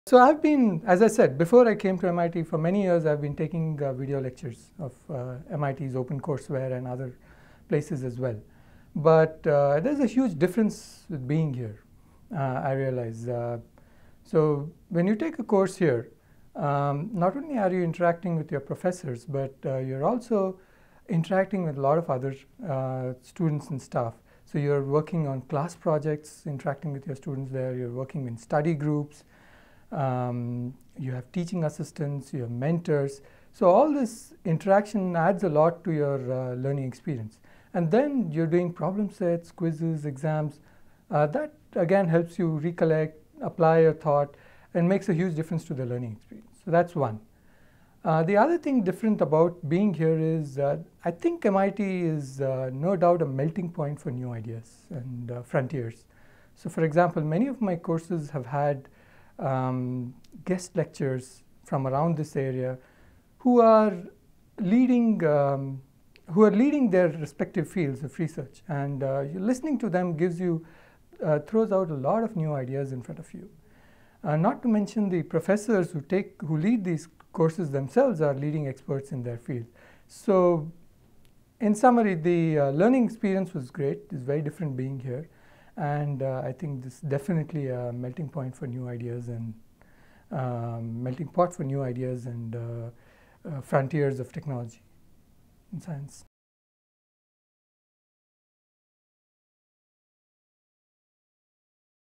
Student Perspective from India -- MIT Advanced Study Program Fellows
Learn more about registering for the Advanced Study Program for professionals at MIT by visiting

Many students are drawn to MIT Professional Education's Advanced Study Program as a way to bridge the gap between undergraduate education and graduate school. Dalesh Dharamshi, an Advanced Study Program Fellow, discusses why he chose the MIT Advanced Study program. From guest lecturers and teaching assistants to study groups and class projects, there is a wealth of knowledgeable people to learn from.